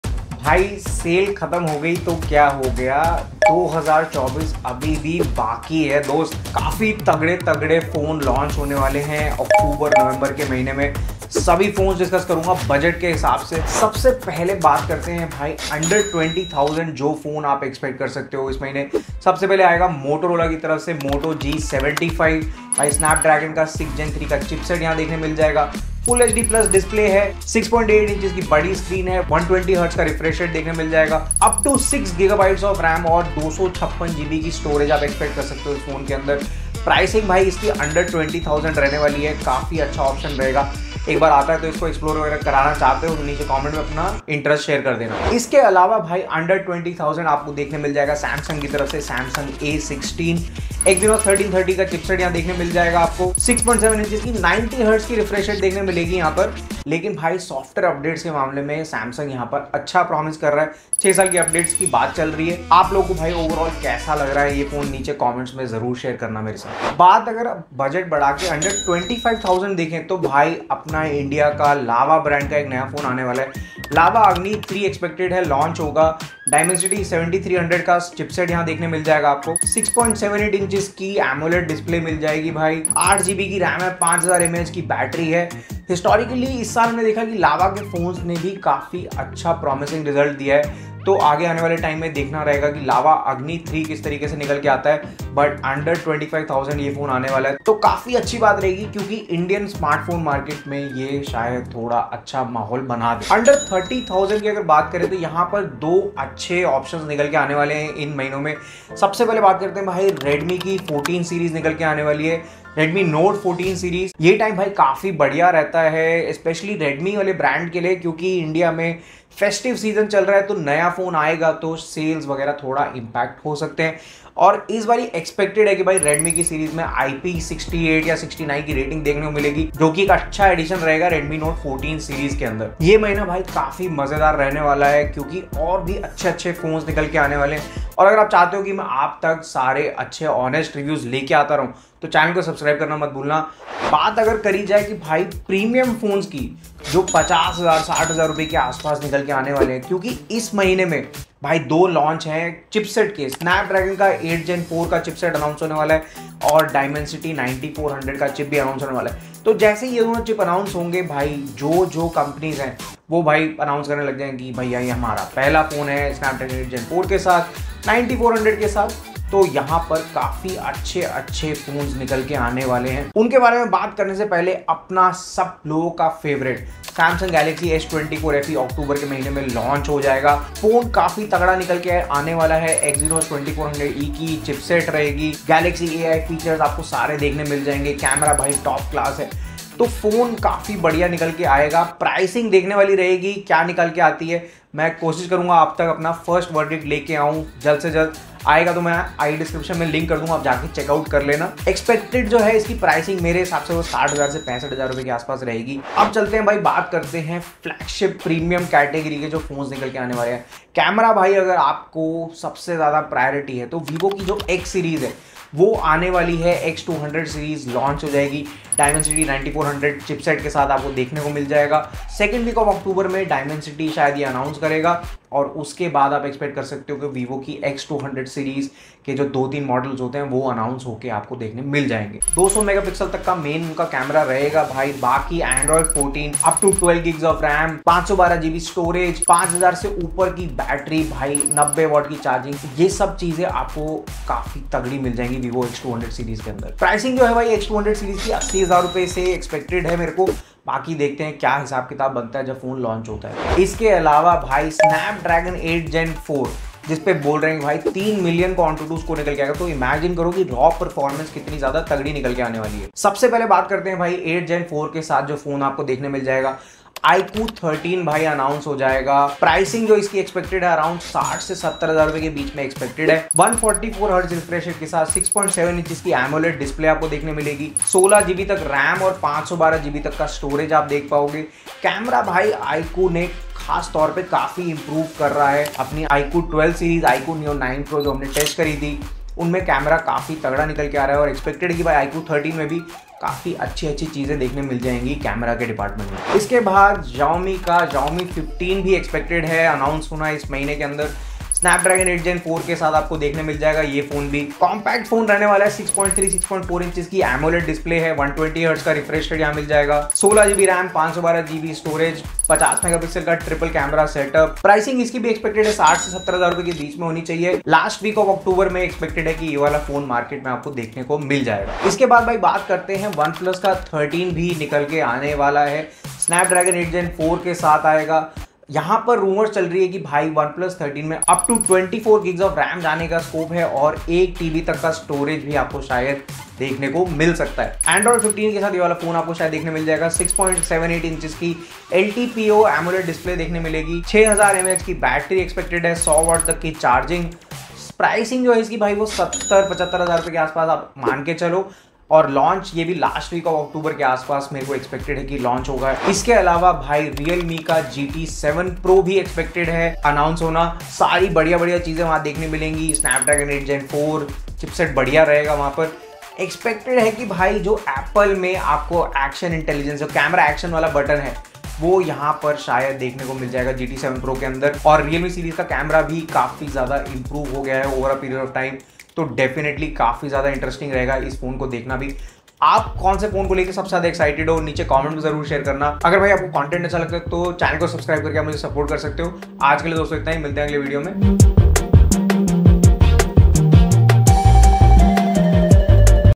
5+ Upcoming Smartphone Launches Under 30000 in October 2024
Dosto,

October is a very exciting month as a lot of new phones are expected to launch. In today's video, I try to share some of the best phone launches at every budget in the month of October.